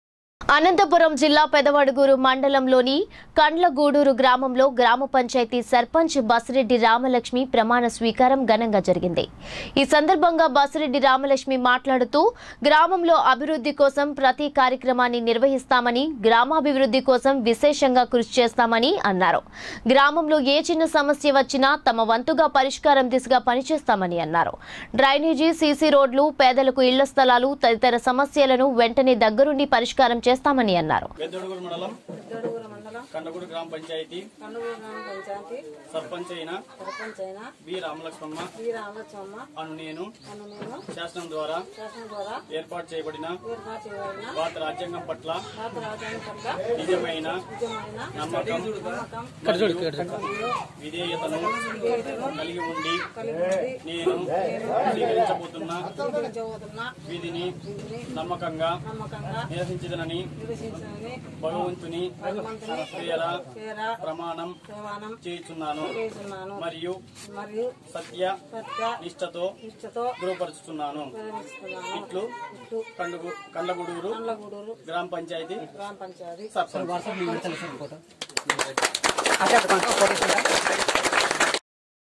పెద్దవడుగూరులో కండ్లగూడూరు పంచాయతీ సర్పంచ్ బసిరెడ్డి రామలక్ష్మి ప్రమాణ స్వీకారం...
@truthnewstelugu Kandlagudur Panchayat Sarpanch Basireddy Ramalakshmi was sworn in at Paddavaduguru ... # TRUTHNEWS